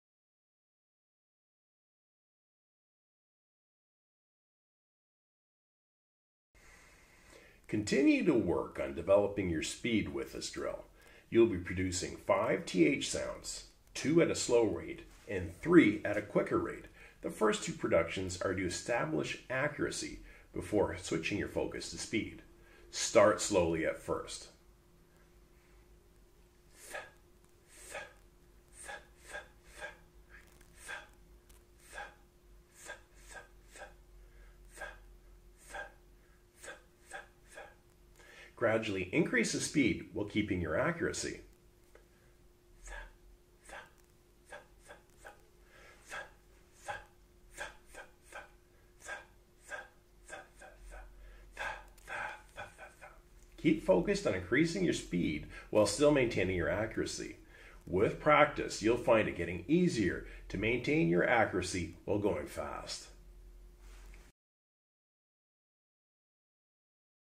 Fast drill - Making the /th/ sound
How to make the /th/ sound using the fast drill. Useful for speech therapists, parents and educators who are working with children on speech sounds.
This video is used in my "Making the /th/ sound tool".